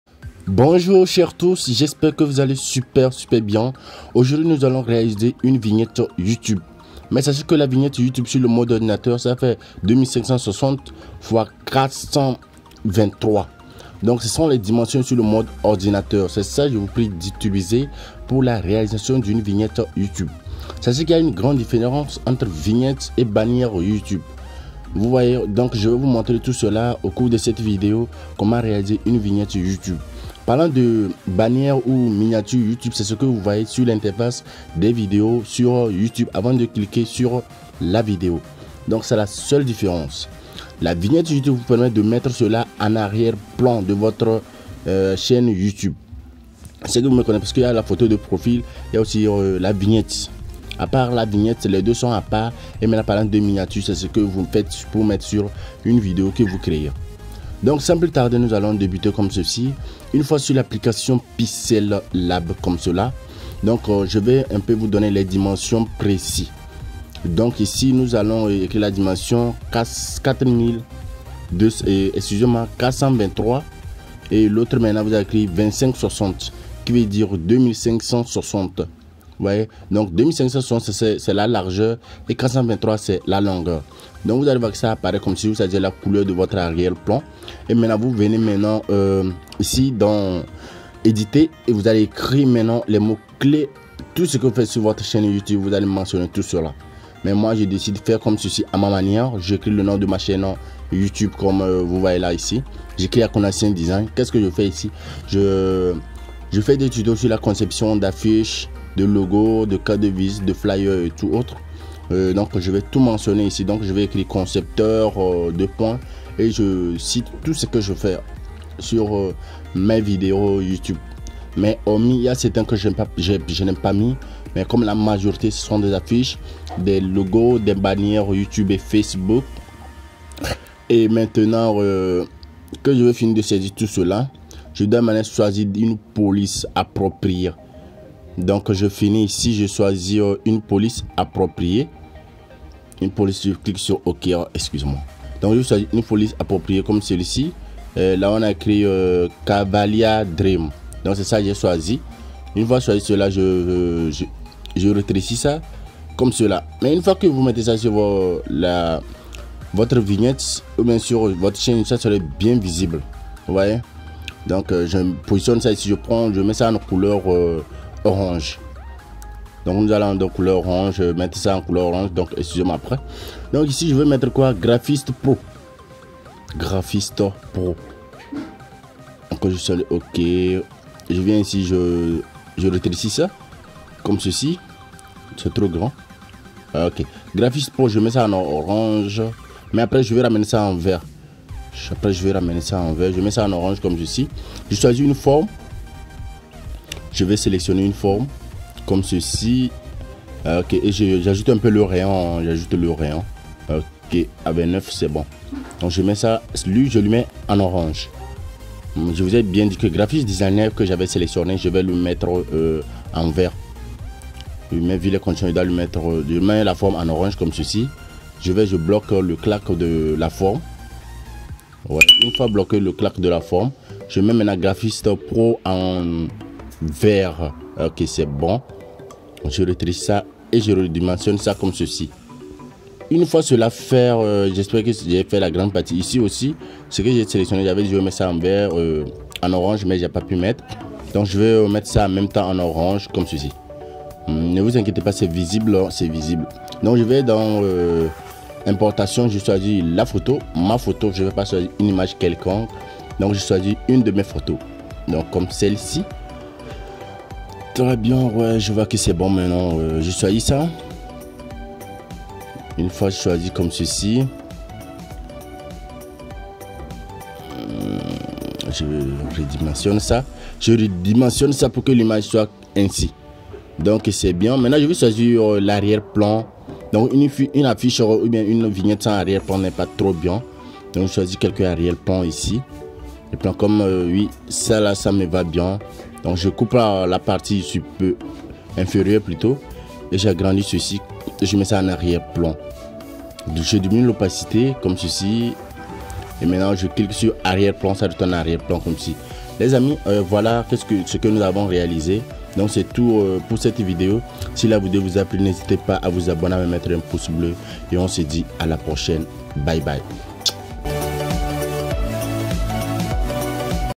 comment réaliser une BANNIÈRE YOUTUBE sur téléphone pour sa chaîne Youtube ? |pixellab
Je vous montre dans cette vidéo une nouvelle stratégie pour vous permettre de mettre une bannière ou de réaliser une bannière avec un téléphone mobile

 Je vous recommande vivement les liens ci-dessous

Merci 😘😘